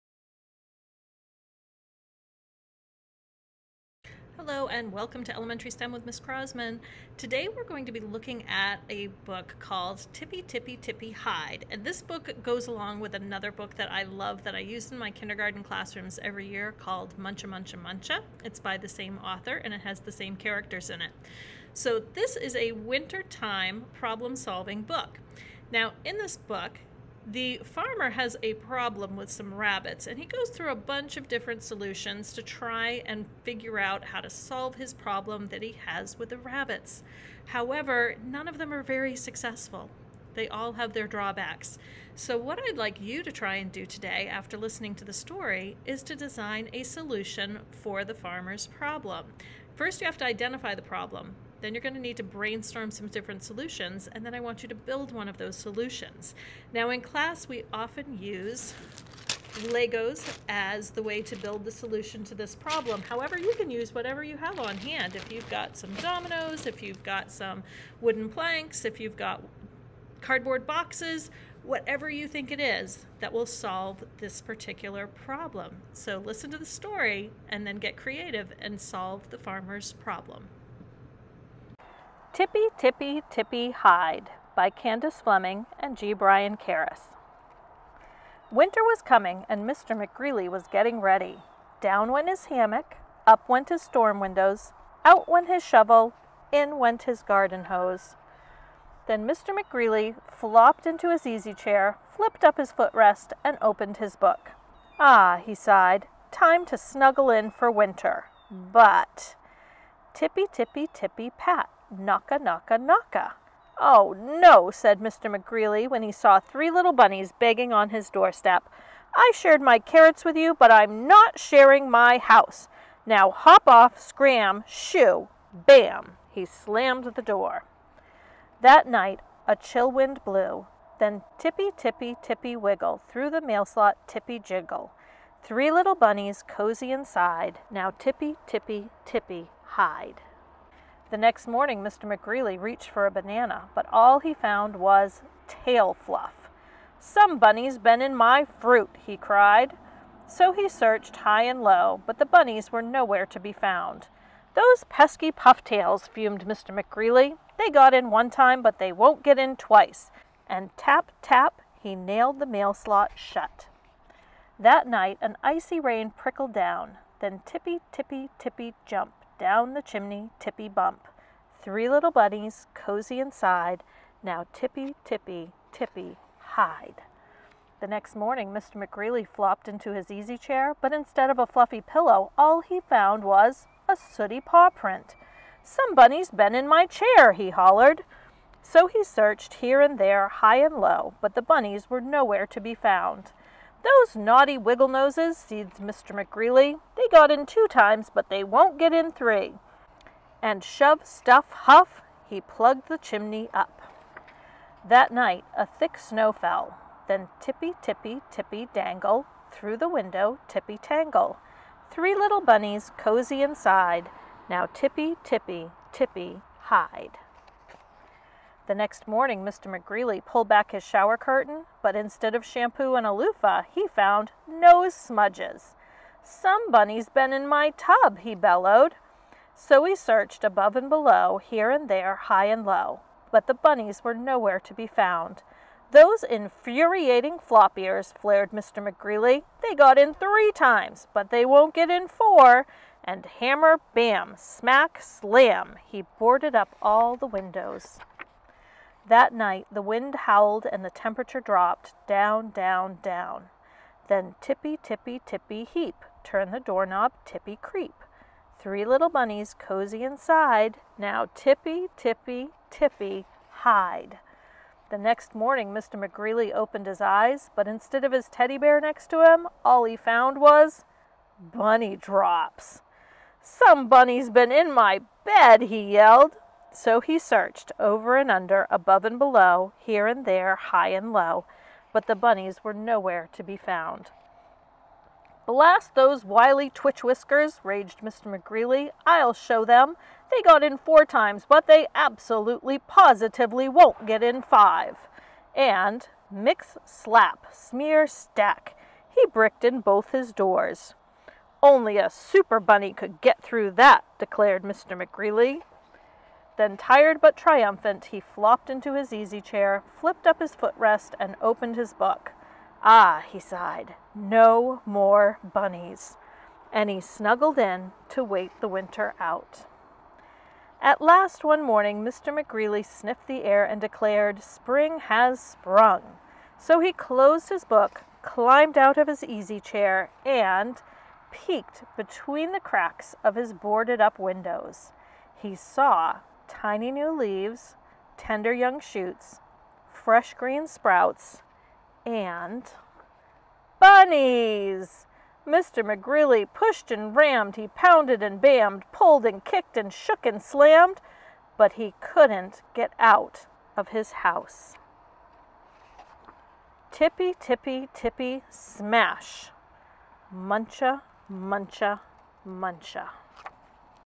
Tippy Tippy Tippy Hide! Read Aloud & STEM Challenge
Figure out the farmer's problem and design a solution to help him! This story is a winter version of my Muncha Muncha Muncha STEM challenge.